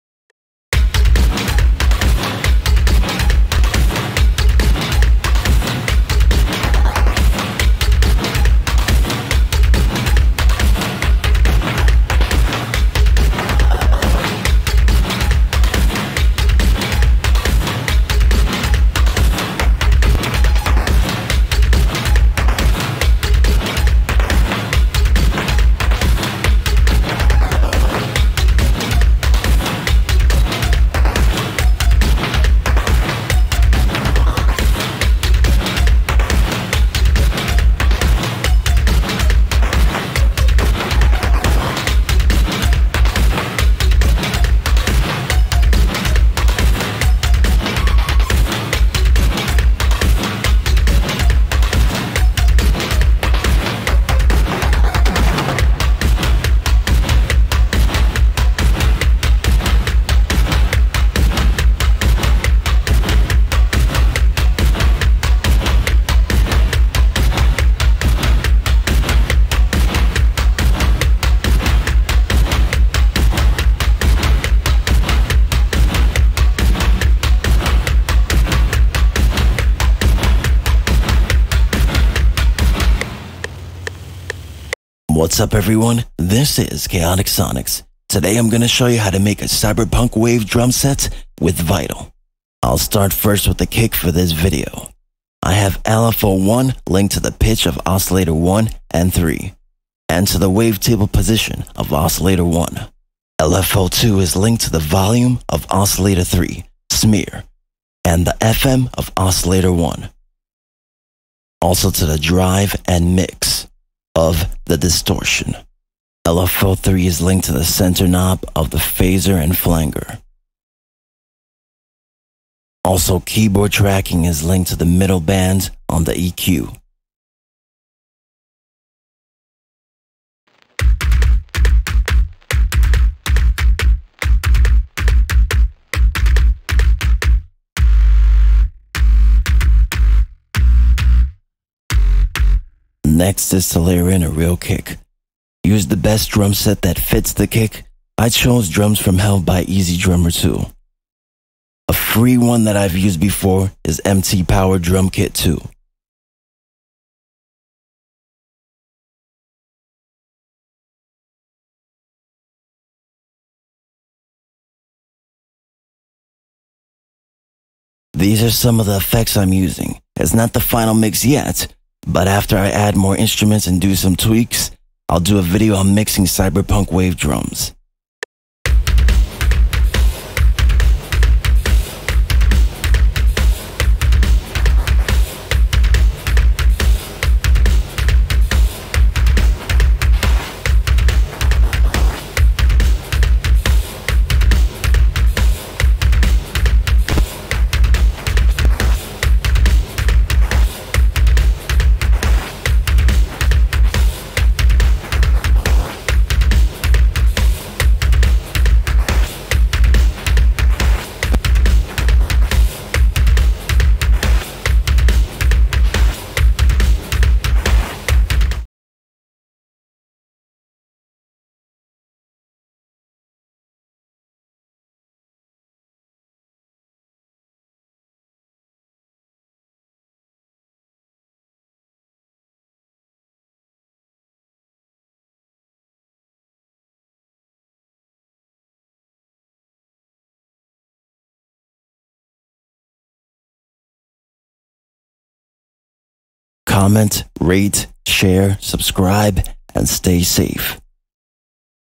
CyberPunkWave Drumset 1 with Vital By Chaoticsonics : The Kick
A Tutorial on making a Cyberpunkwave Drumset with Vital in FL Studios 20 . This video is on the kick. Next Video will be the Hi hats.